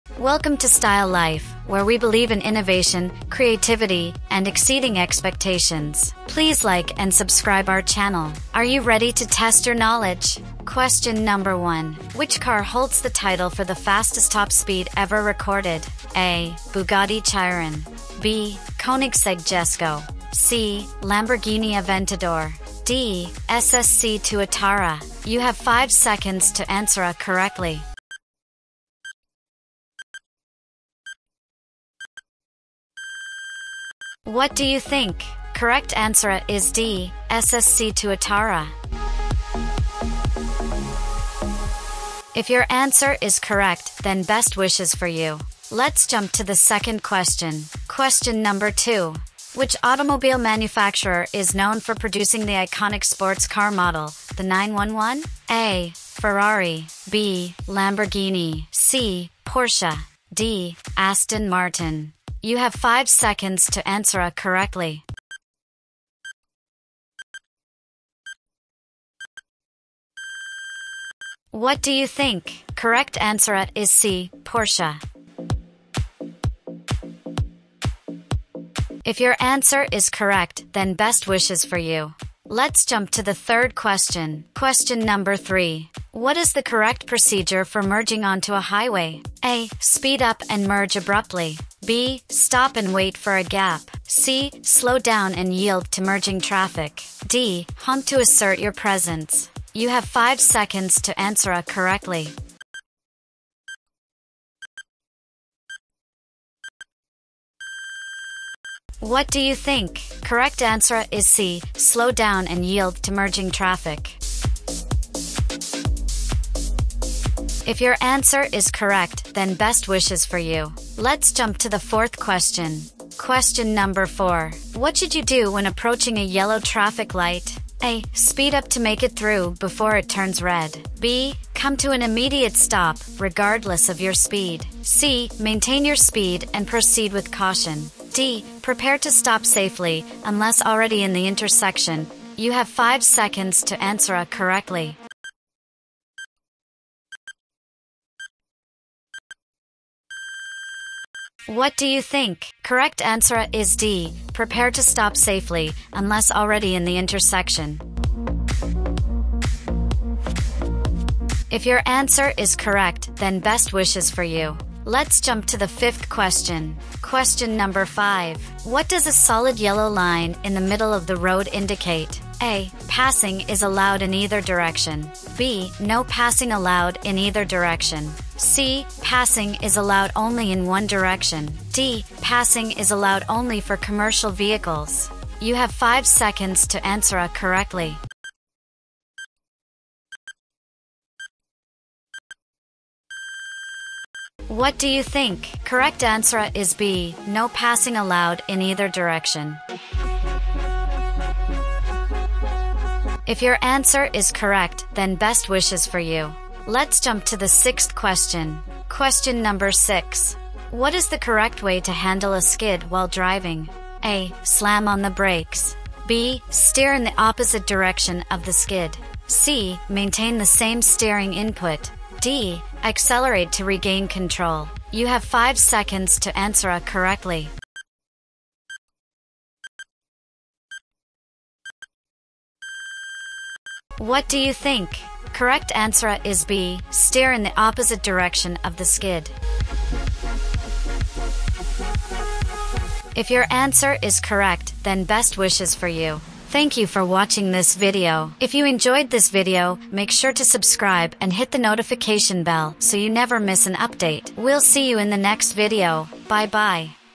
Revving minds car and car driving quiz challenge / car quiz 🚕🚘
Welcome to our Car and Car Driving Related Quiz! Buckle up and test your knowledge in this exciting quiz that will challenge your understanding of cars, driving techniques, and automotive trivia. Join us as we dive into a variety of questions and engage in a fun and educational journey through the world of automobiles. From identifying iconic car models to mastering road signs and regulations, this quiz is designed to entertain and educate car enthusiasts of all levels. So, rev your engines, put your thinking caps on, and let's embark on this thrilling automotive quiz adventure together!